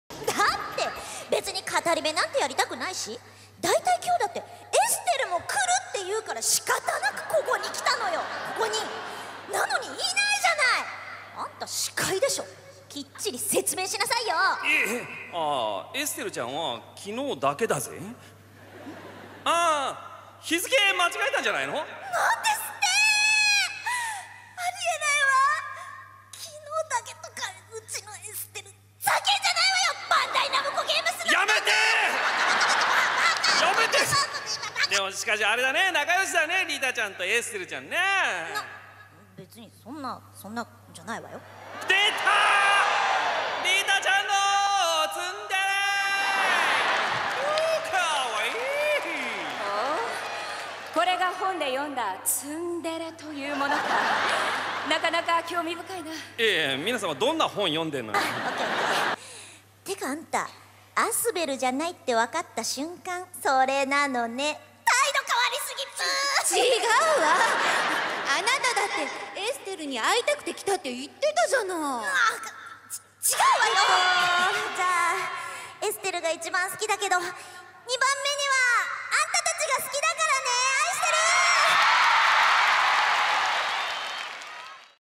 tales of ristelle
basically tales of festival 2012 summed up

i s2g i spend my time subbing the gayest things
i thought about subbing the whole thing but you know it's like more than 3 hours long so fuck it